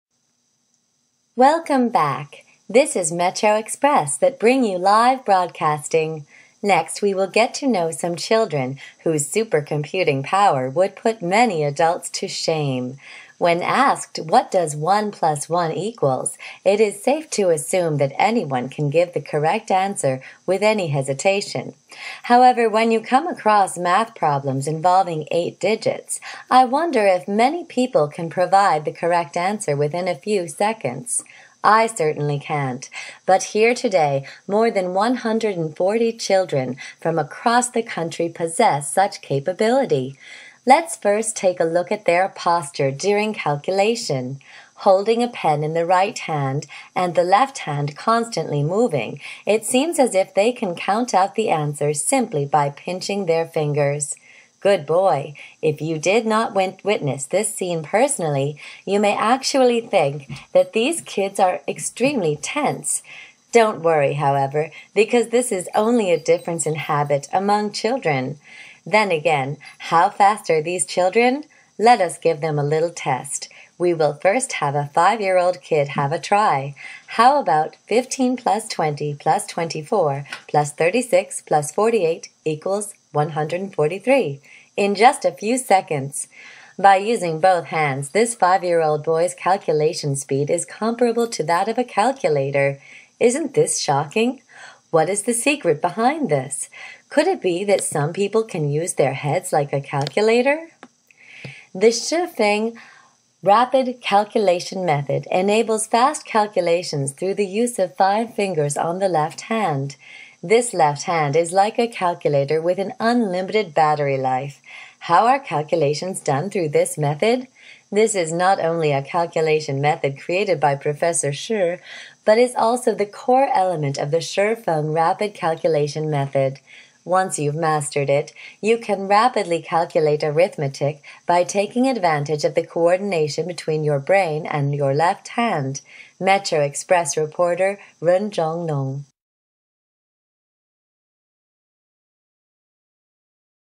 2012 ShiFengShou Rapid calculation
"Shi Fengshou Rapid Calculation" , once praised by the UNESCO as a miracle in the history of science education, that it had significant influence in human brain development, and that it should be promoted thoughout the world. At present, it has been a popular educational tool among American and Asian areas.